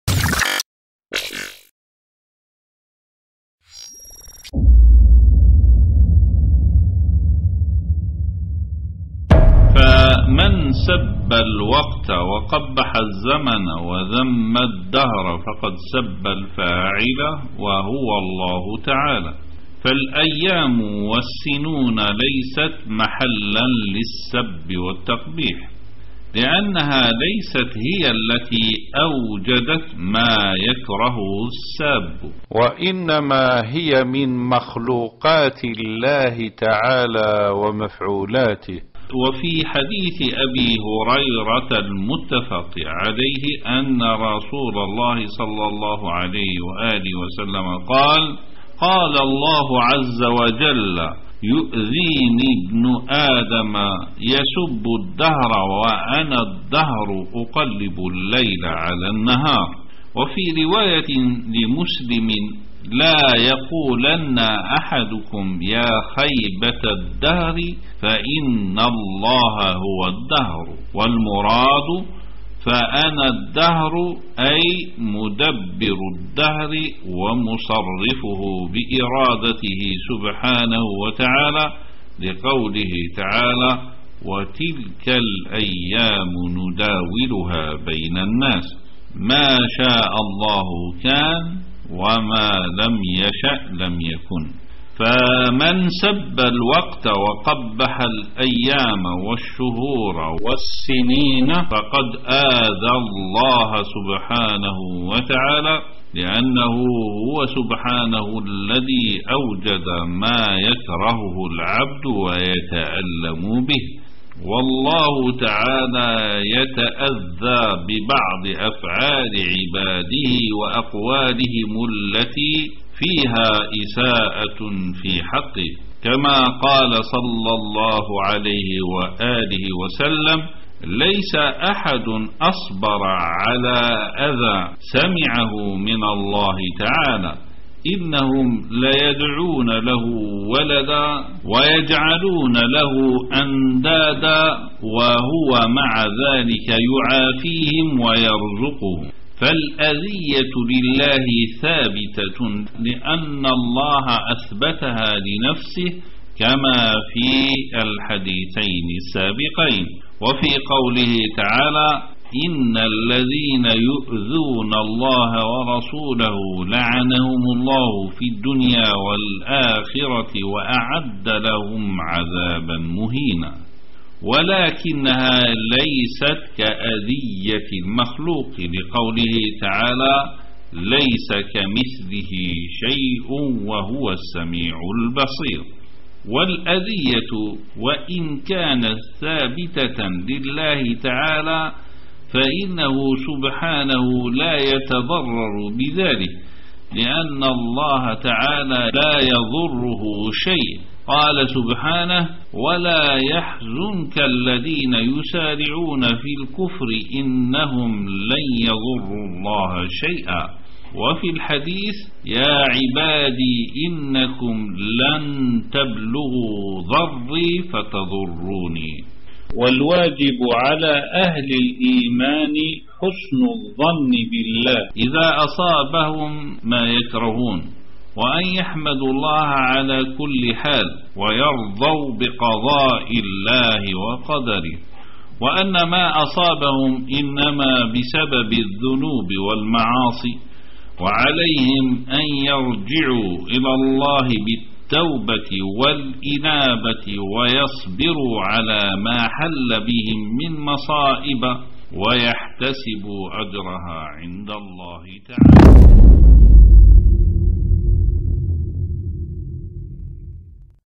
Cursing 2020 Is Cursing Allah ﷻ┇Shaykh Muhammad Ali Firkous
Shaykh Muhammad Ali Firkous reminds the Muslims that cursing time in any capacity is tantamount to cursing Allah ﷻ Himself as Allah has said, "I Am Time."